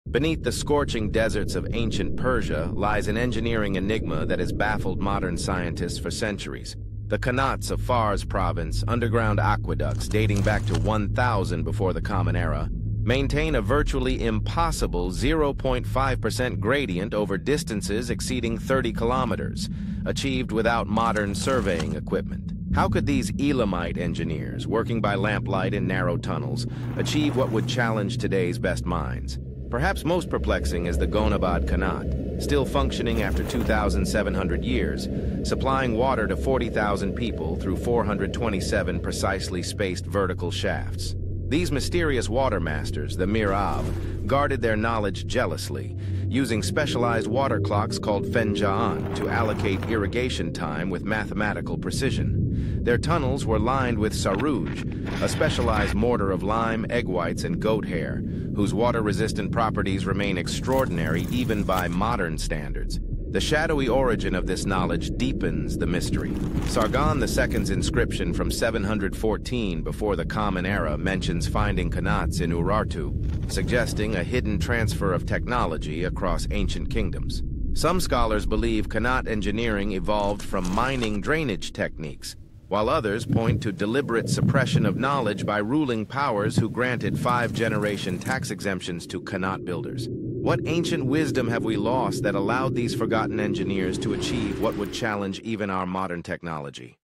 Persian Water WIZARDS; Ancient Engineering DEFIES Belief!
Today on Secret History, we explore the ancient Persian qanatsingenious underground aqueducts engineered with flawless gradients over vast distances without modern tools. Discover qanats over 2,700 years old, like the Gonabad qanat featuring 427 vertical shafts, still supplying thousands with water through mathematically precise channels. Unveil the secrets of the mir'ab water masters who employed specialized water clocks and crafted sarooj, a water-resistant mortar defying modern science. Dive into inscriptions from 714 BCE by Sargon II hinting at hidden knowledge transfers and possible deliberate suppression of this technology by ancient rulers granting tax exemptions to control water. What ancient engineering marvels and lost wisdom still challenge our modern technology?